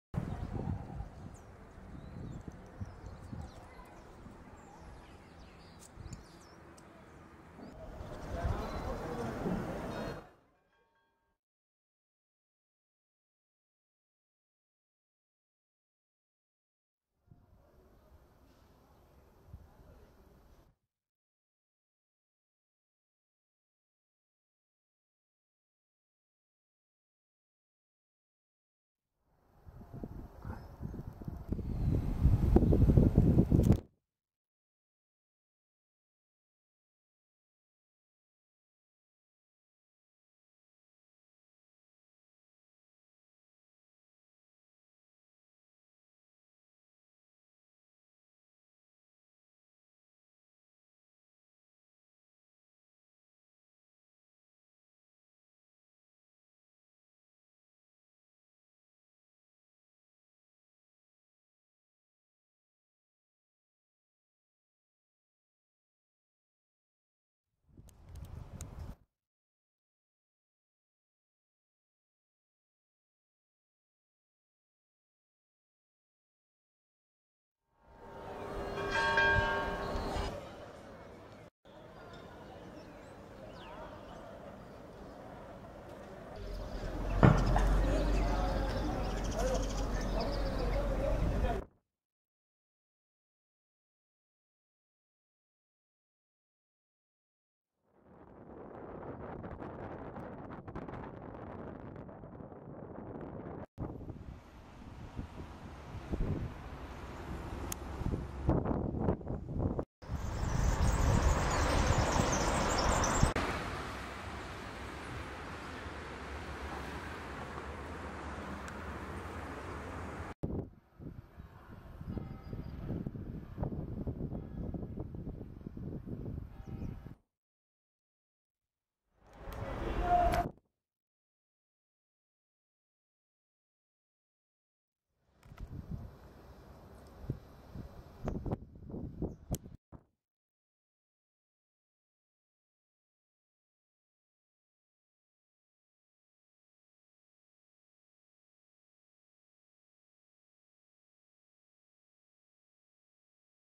Cáceres, Spain: Views of the city - Vistas de la ciudad
Interesting views of the city of Caceres can be obtained from the towers of the Church of  San Francisco Javier and the cathedral. The whole old city and the surroundings are commanmded from any of them. From the east side of the city there also beautiful views of the city walls and towers.
Desde lo alto de las torres de la Iglesia de San Francisco Javier y de la Catedral se obtienen buenas vistas del casco viejo de caceres y sus alrededores. Del laado este de la ciudad hay biuenas vistas de las murallas y sus torres.